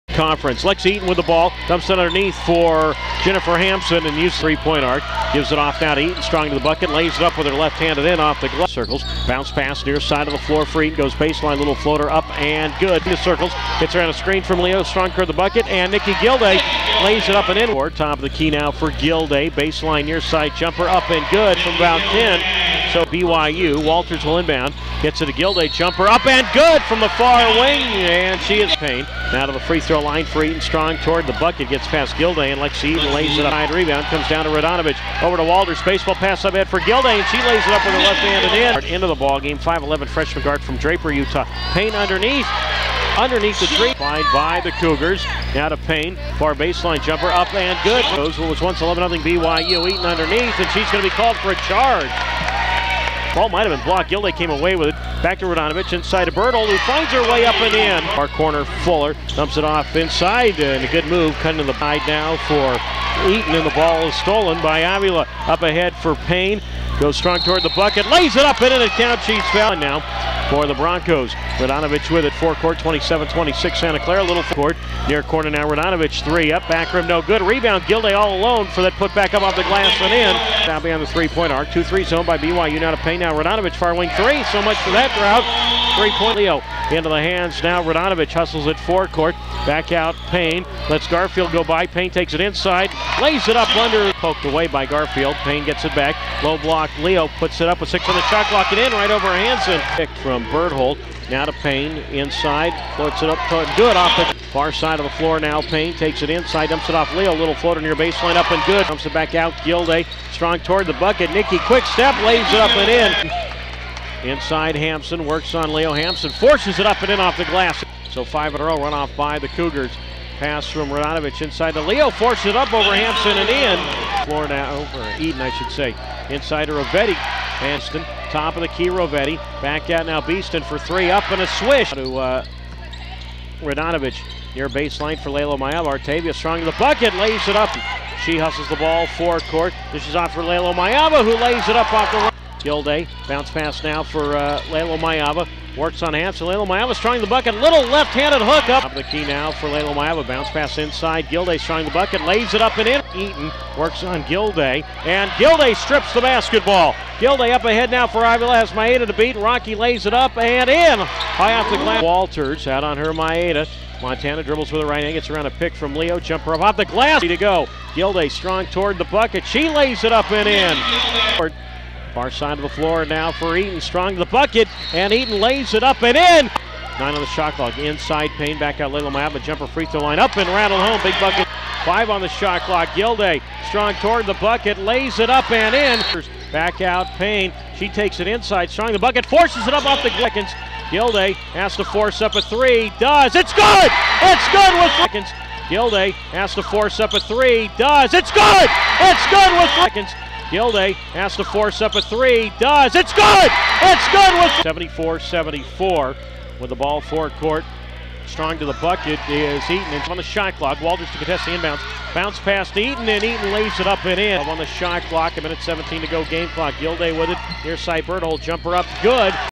Santa Clara Women's Basketball vs. BYU
In its third overtime game of the season, Santa Clara women's basketball lost to BYU by a final of 87-80 on Thu., Feb. 6, 2014 at Santa Clara's Leavey Center. The loss puts the Broncos at 7-16 overall and 3-8 in WCC action while the Cougars are now 18-5 overall and 8-4 in conference play.